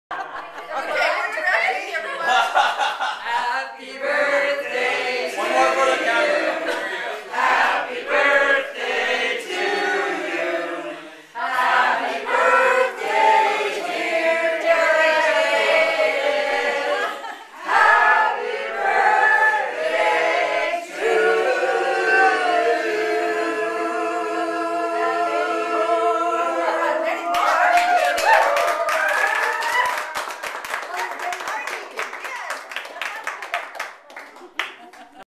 Happy Birthday Drumming Circle Party Salmon Arm BC Canada
Happy Birthday for David and Carolyn Part at Goldie Denise and James Wilson Salmon Arm, BC Canada  VID00026.mp4